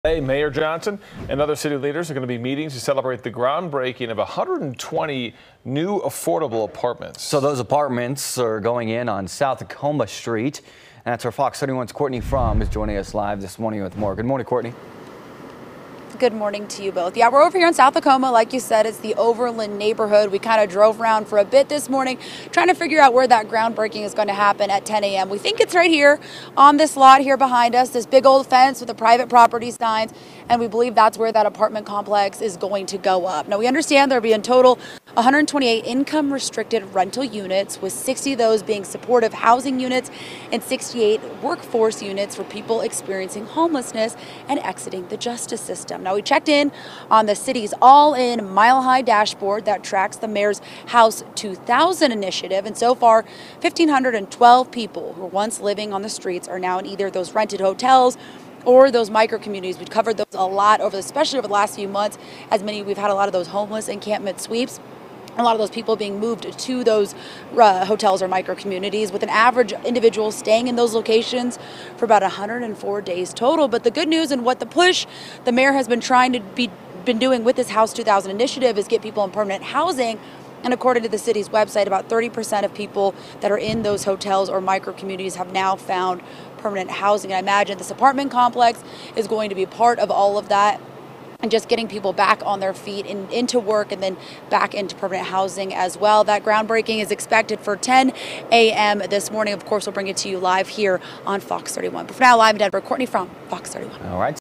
120 affordable apartments are coming to Denver
120 new affordable apartments are coming to Denver. FOX31's Courtney Fromm is detailing the specifics of the lot.